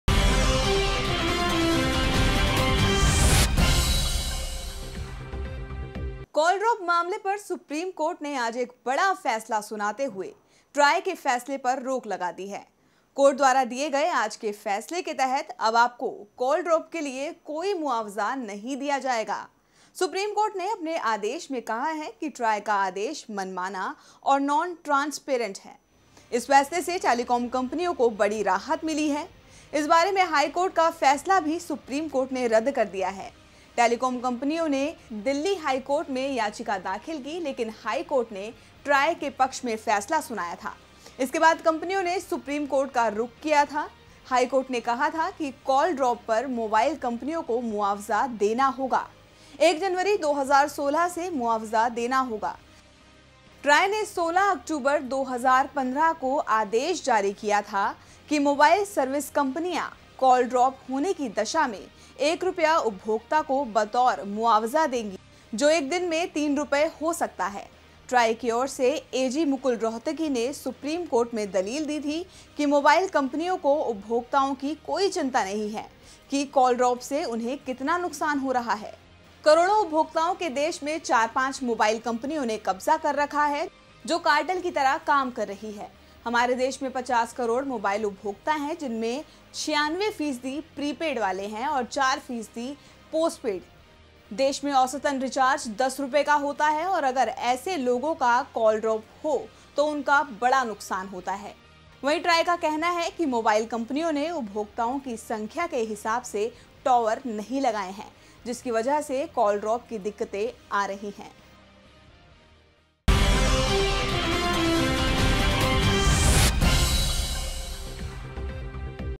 DBLIVE|11MA|2016 |CALL DROP कॉल ड्रॉप मामले पर सुप्रीम कोर्ट का फैसला
कॉल ड्रॉप मामले पर सुप्रीम कोर्ट ने आज एक बड़ा फैसला सुनाते हुए TRAI के फ़ैसले पर रोक लगा दी है। कोर्ट द्वारा दिए गए आज के फैसले के तहत अब आपको कॉल ड्रॉप के लिए कोई मुआवजा नहीं दिया जाएगा। सुप्रीम कोर्ट ने अपने आदेश में कहा है कि ट्राई का आदेश मनमाना और नॉन ट्रांसपेरेंट है।इस फैसले से टेलिकॉम कम्पनियों को बड़ी राहत मिली है। इस बारे में हाईकोर्ट का फैसला भी सुप्रीम कोर्ट ने रद्द कर दिया है। टेलिकॉम कंपनियों ने दिल्ली हाईकोर्ट में याचिका दाखिल की लेकिन हाईकोर्ट ने ट्राई के पक्ष में फैसला सुनाया था। इसके बाद कंपनियों ने सुप्रीम कोर्ट का रुख किया था। हाई कोर्ट ने कहा था कि कॉल ड्राप पर मोबाइल कंपनियों को मुआवजा देना होगा। 1 जनवरी, 2016 से मुआवजा देना होगा। ट्राई ने 16 अक्टूबर 2015 को आदेश जारी किया था कि मोबाइल सर्विस कंपनियां कॉल ड्राप होने की दशा में 1 रुपया उपभोक्ता को बतौर मुआवजा देंगी जो एक दिन में 3 रुपये हो सकता है।
ट्राई की ओर से AG मुकुल रोहतगी ने सुप्रीम कोर्ट में दलील दी थी कि मोबाइल कंपनियों को उपभोक्ताओं की कोई चिंता नहीं है कि कॉल ड्राप से उन्हें कितना नुकसान होता है। करोड़ों उपभोक्ताओं के देश में 4-5 मोबाइल कंपनियों ने कब्जा कर रखा है जो कार्टेल की तरह काम कर रहीं हैं। हमारे देश में 50 करोड़ मोबाइल उपभोक्ता हैं जिसमें 96 फीसदी प्री पेड वाले हैं और 4 फीसदी पोस्ट पेड हैं। देश में औसतन रीचार्ज 10 रुपये का होता है और अगर ऐसे लोगों का कॉल ड्राप हो तो उनका बड़ा नुकसान होता है। वहीं ट्राई का कहना है कि मोबाइल कंपनियों ने उपभोक्ताओं की संख्या के हिसाब से टावर नहीं लगाए हैं जिसकी वजह से कॉल ड्राप की दिक्कतें आ रही हैं।